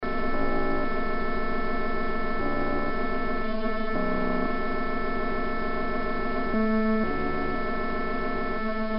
Tape Storage Machine Hum Buzz Sound Effect 1
Office ambience sound, tape machine.